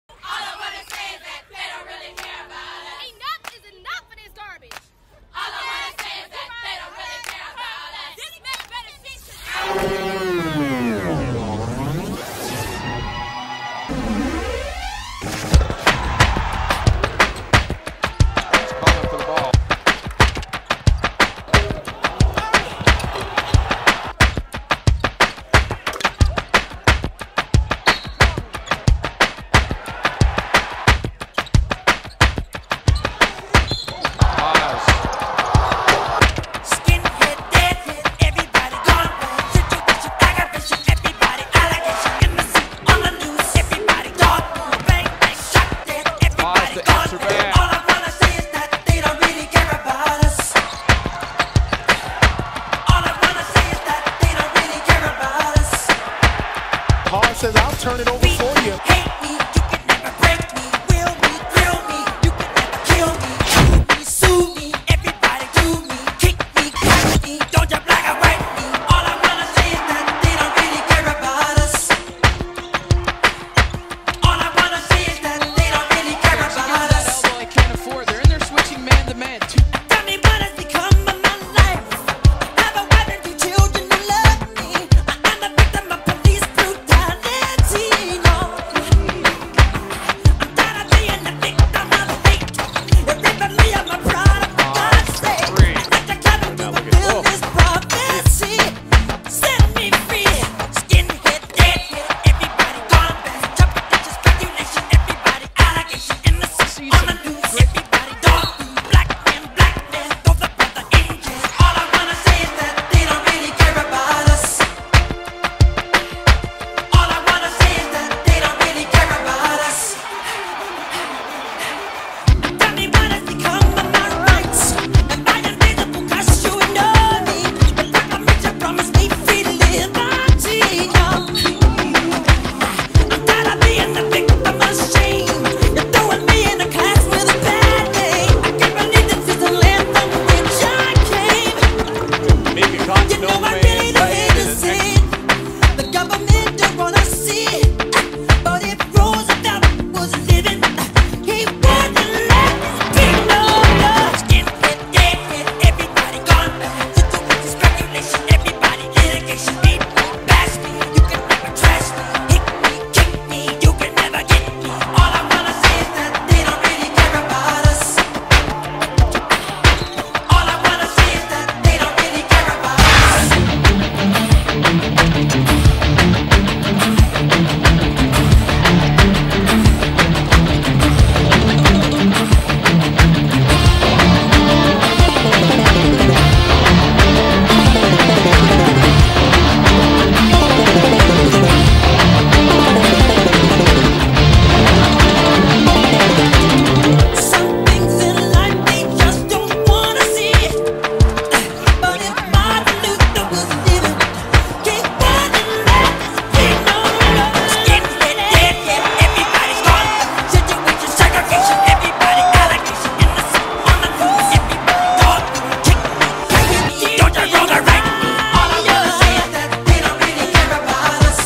Tyler Haws Season 2013 2014
Contains all of the highlights Tyler Haws produced during the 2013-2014 season